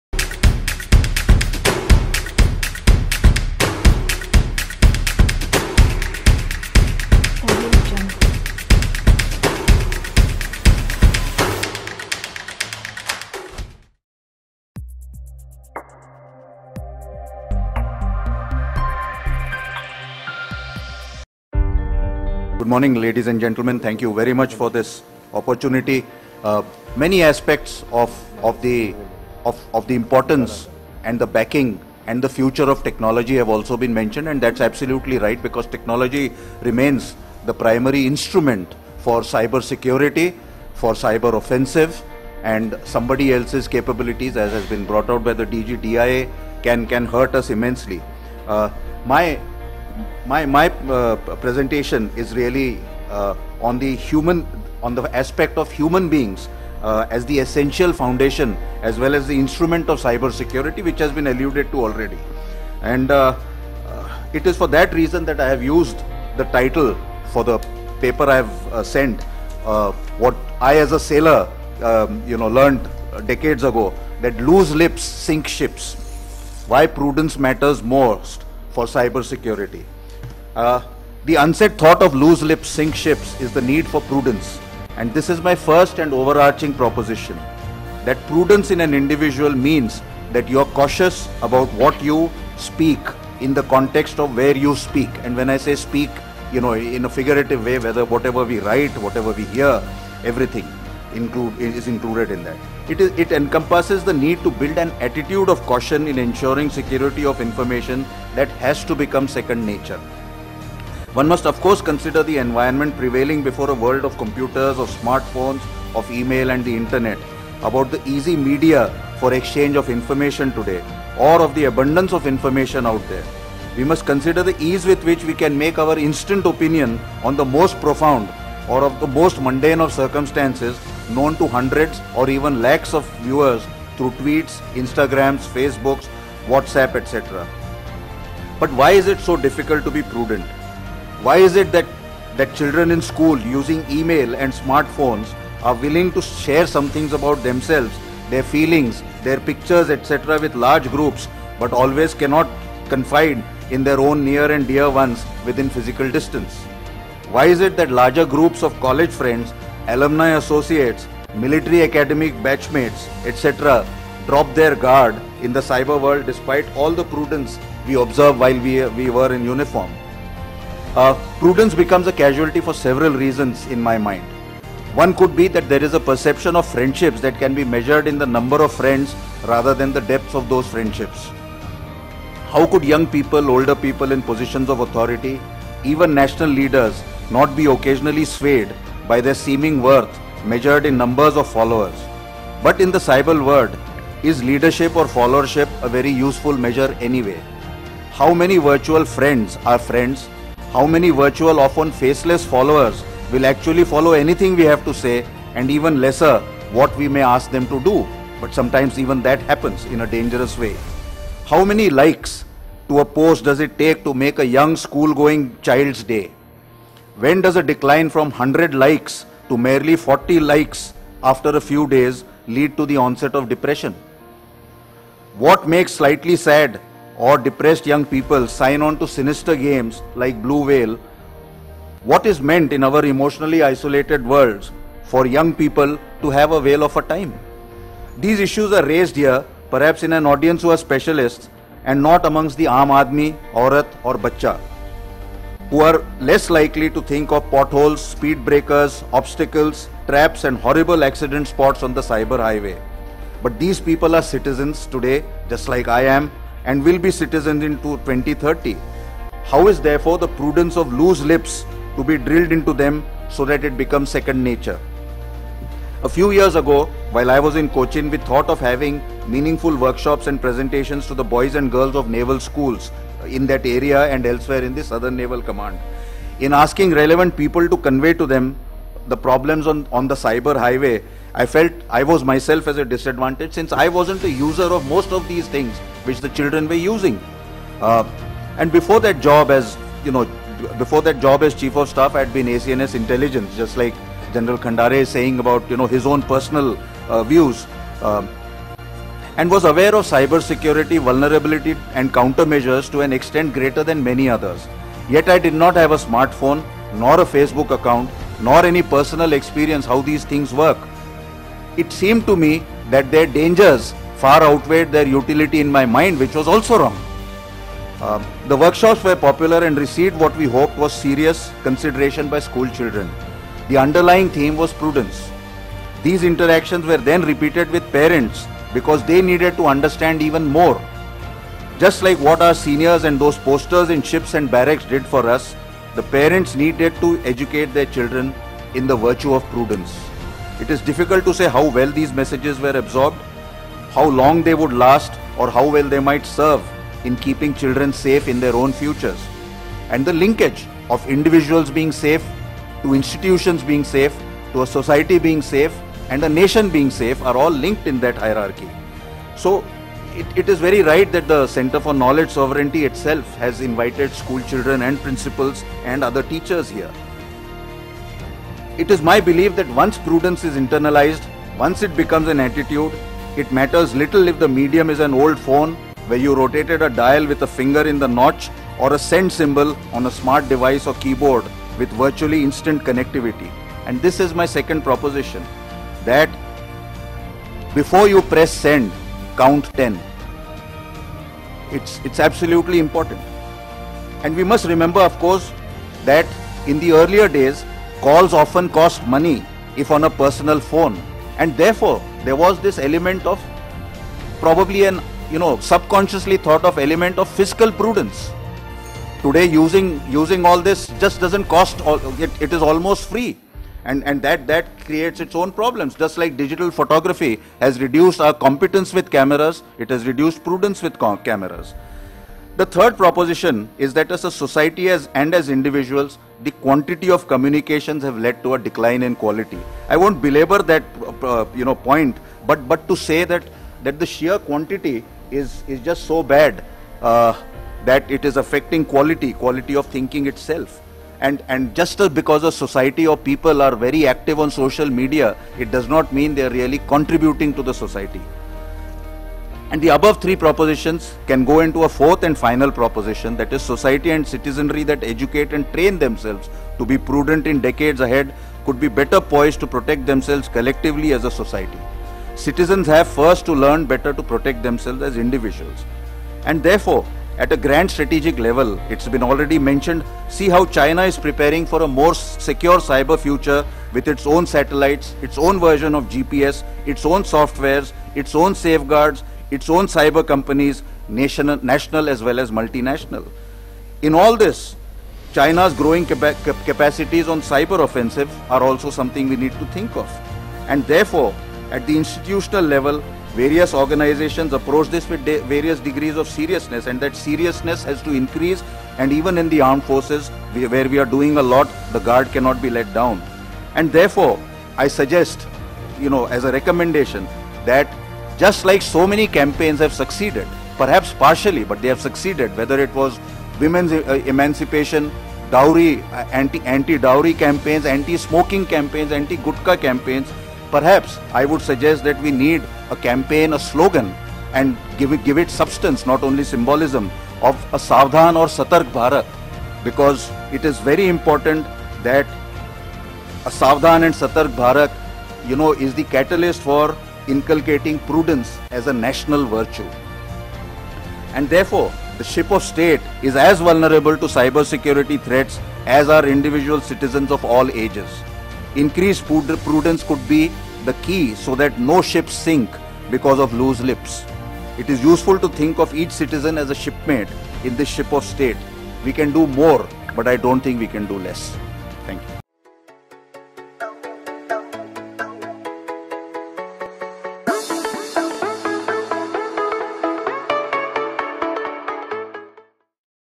Rear Admiral S Y Shrikhande, AVSM (Retd)
'Technology remains the primary instrument for cyber security': Rear Admiral S Y Shrikhande, AVSM (Retd)